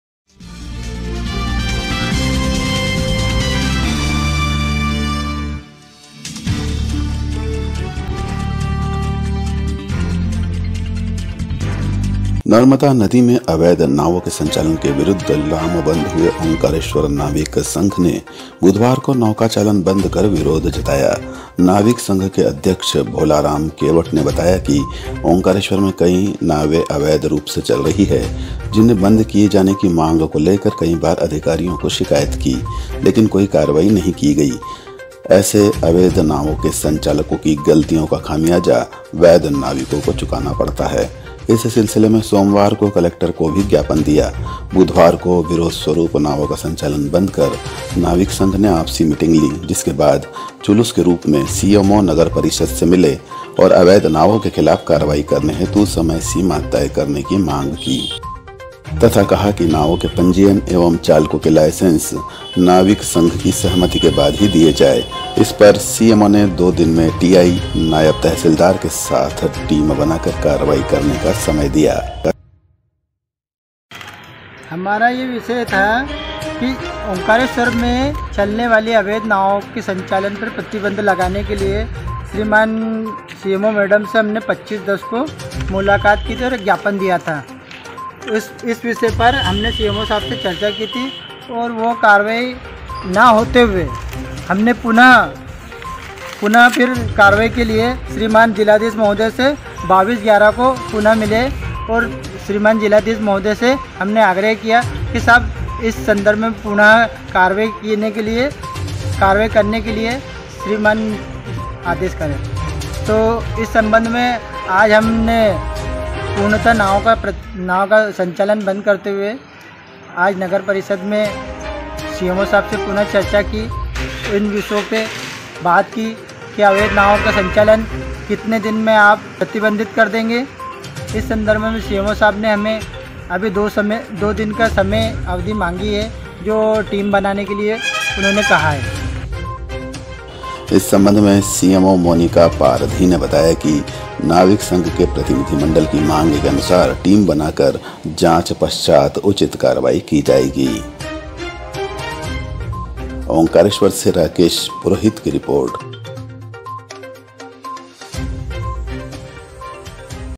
ओम्कारेश्वर - धड़ल्ले से चल रही अवैध नावे बंद करने की मांग MP NEWS NETWORK OMKARESHWAR NEWS
नर्मदा नदी में अवैध नावों के संचालन के विरुद्ध लामबंद हुए ओम्कारेश्वर नाविक संघ ने बुधवार को नौका चालन बंद कर विरोध जताया. नाविक संघ के अध्यक्ष भोलाराम केवट ने बताया कि  ओम्कारेश्वर में कई अवैध नावे धड़ल्ले से चल रही है. जिन्हें बंद किये जाने की मांग को लेकर कई बार अधिकारियों को शिकायत की. किंतु कोई कार्रवाई नहीं हुई.उनकी गलतियों का खामियाजा, वैध नाविकों को चुकाना पड़ता है.